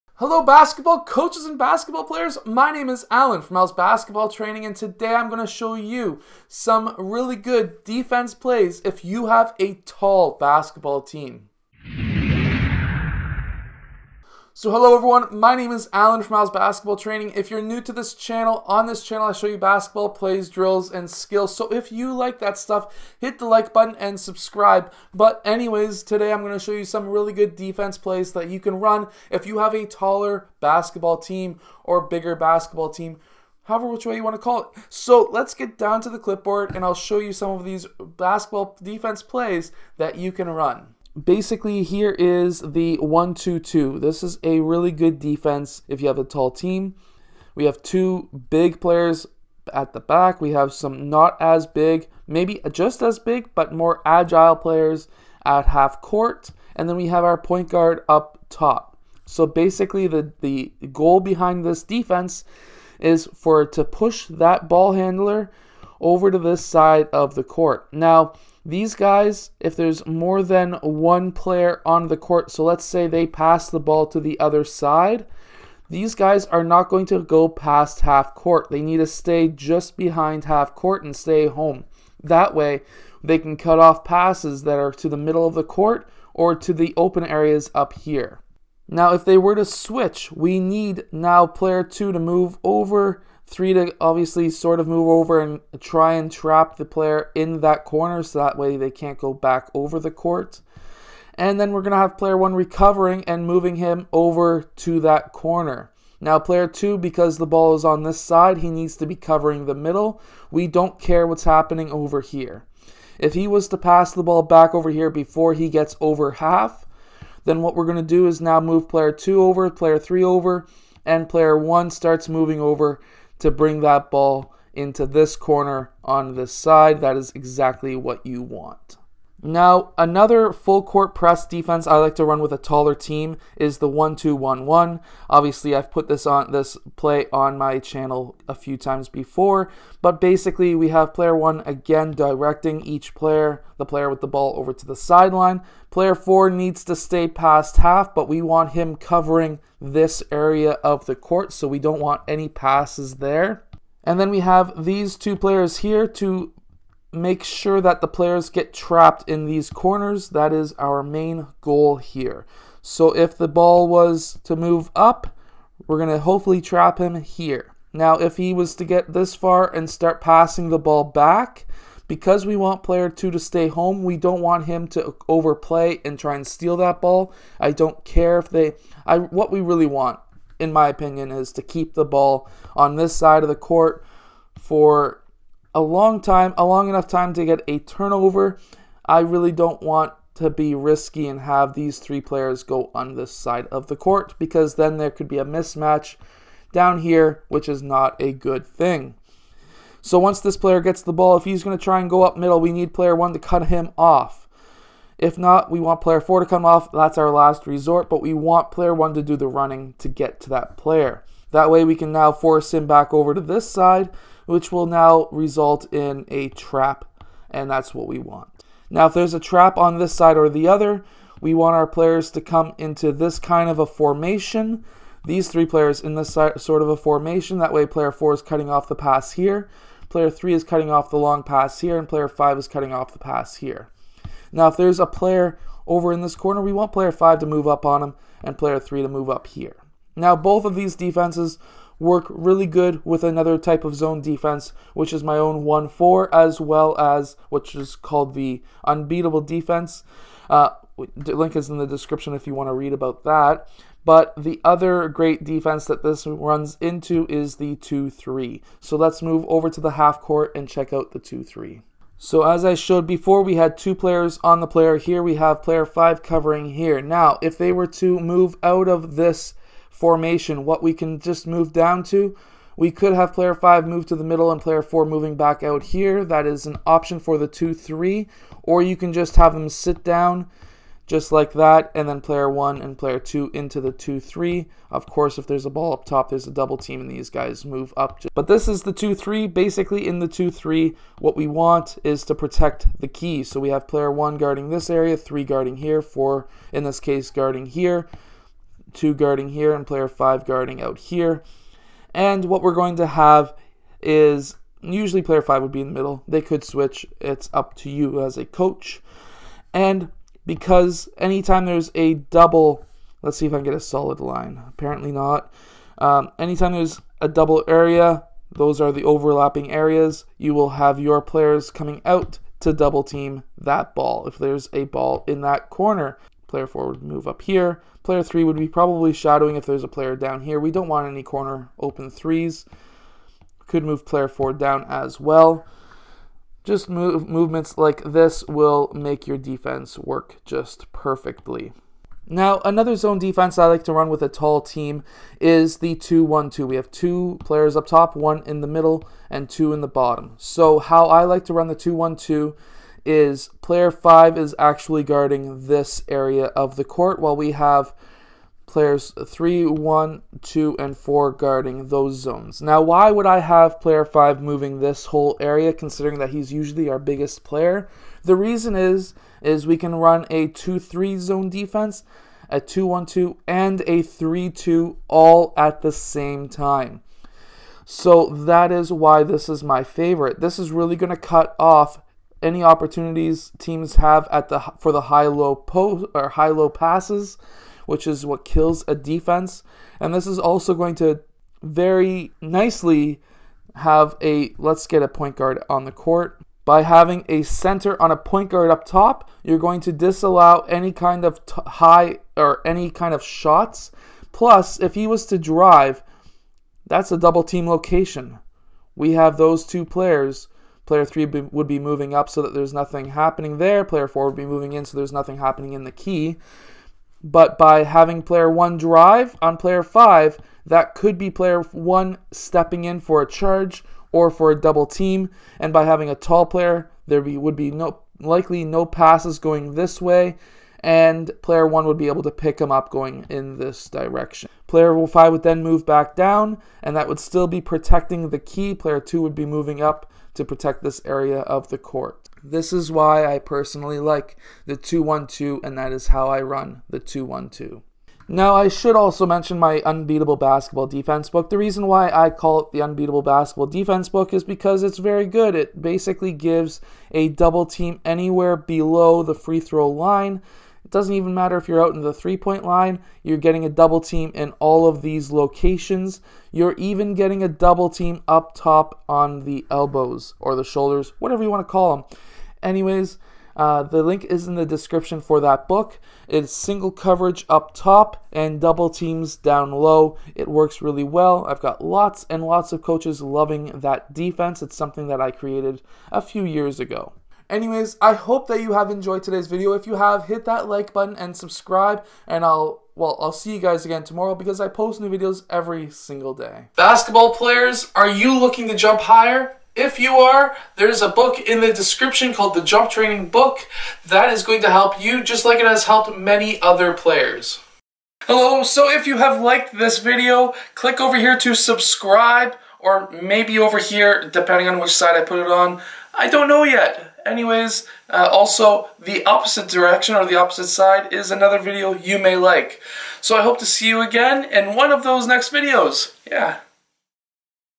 Best Defense Plays For Tall Basketball Teams
Best Defense Plays For Tall Basketball Teams, if you have a tall Basketball team, these are the defense play I would run with them. With a full court press defense I would run a 1-2-2 full court press and a 1-3-1-1 Full court press as those formations work really good by keeping your big players back home in the key. In the Half court I would run a 2-3 zone, and 2-1-2 zone defense so you can keep your big players around the basket.